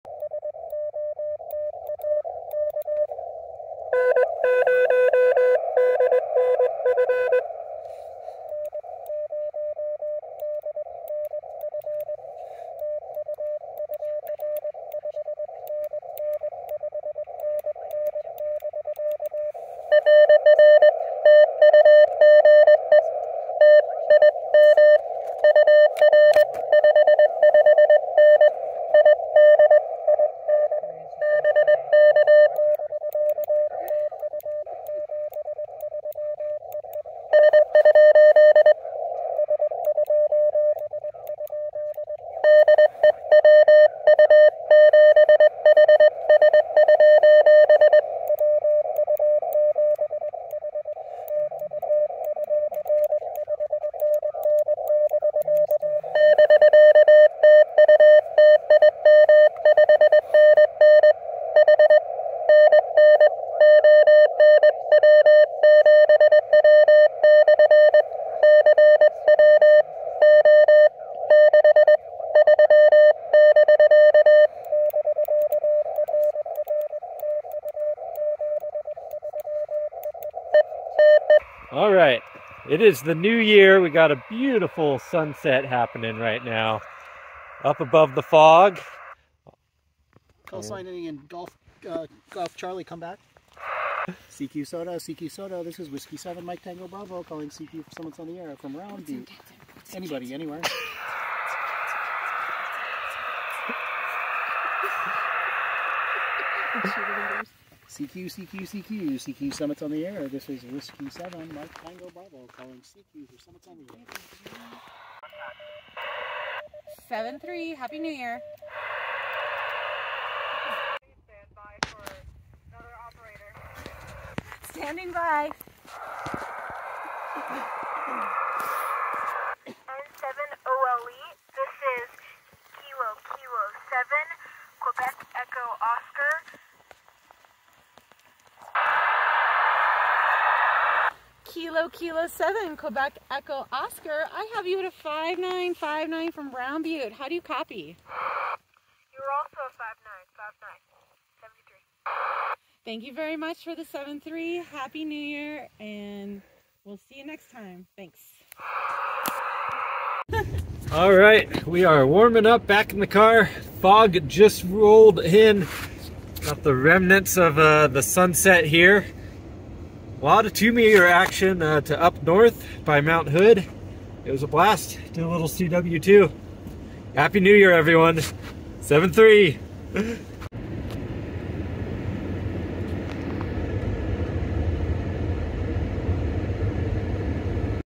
New Years Eve UTC Rollover Activation! Happy SOTA New Year from Central Oregon!
We got on the air just about 2316 UTC time and got a bunch of 2m Summit to Summit QSOs with a crew up on Frog Lake buttes (W7O/CN-106). We had a blast working 2m FM with the yagi and Yaesu FT-90. We also did a little bit of HF, but the 2m was keeping us busy!

The UTC New Year is when the SOTA database resets for the year. So a summit can be activated in both the current and the new year for double the points in one outing! Here in Oregon the rollover happens at 4pm. We got super lucky with great weather and a fantastic sunset from Round Butte.

Microlight QRP Traps available now!